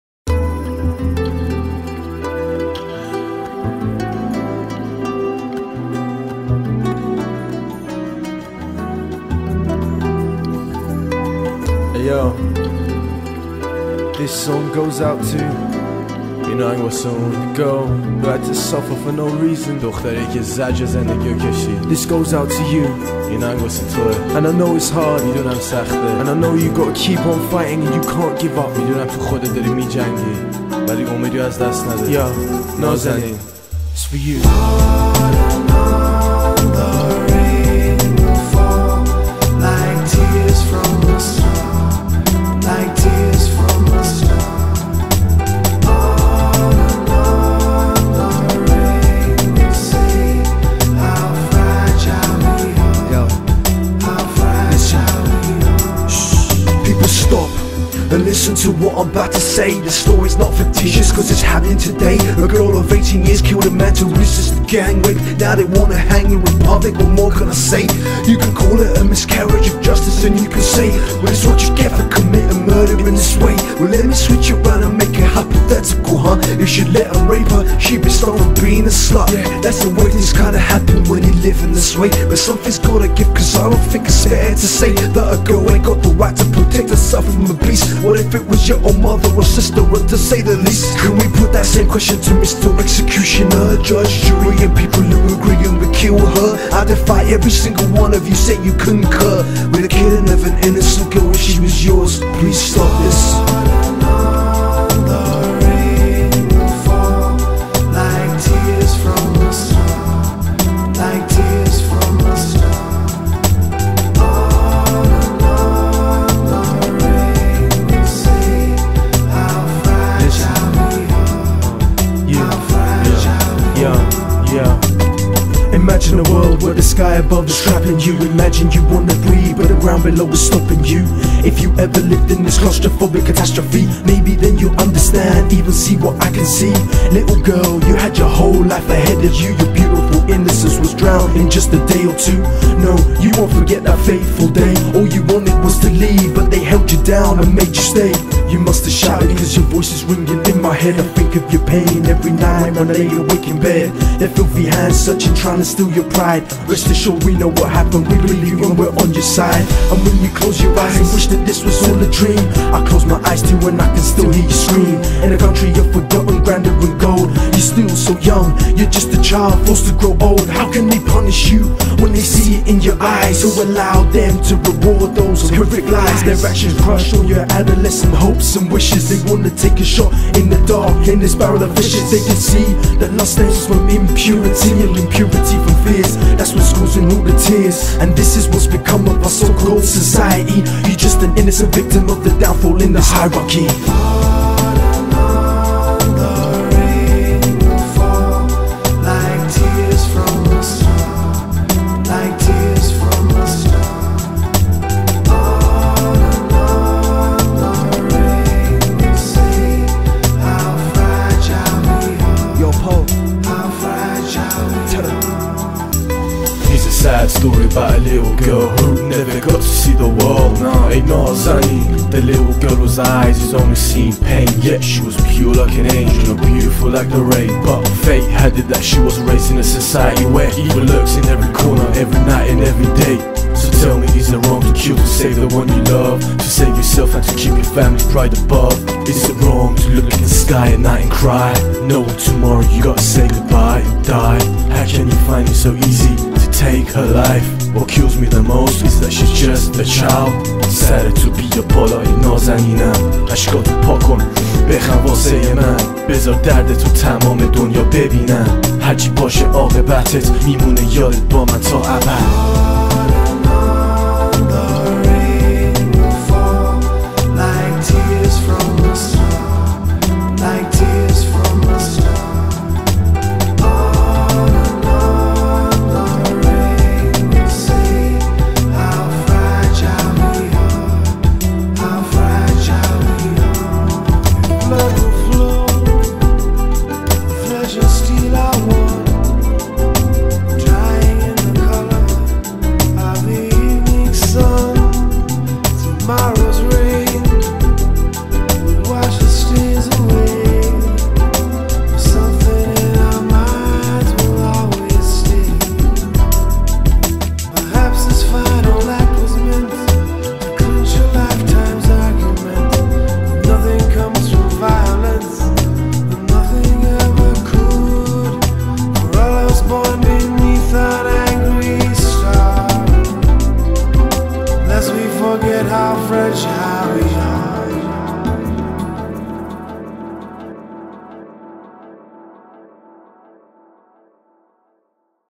SAVE NAZANIN FATEHI
An 18 year old Iranian girl was facing execution for killing in self defense one of the men who were attempting to rape her and her 15 year old niece.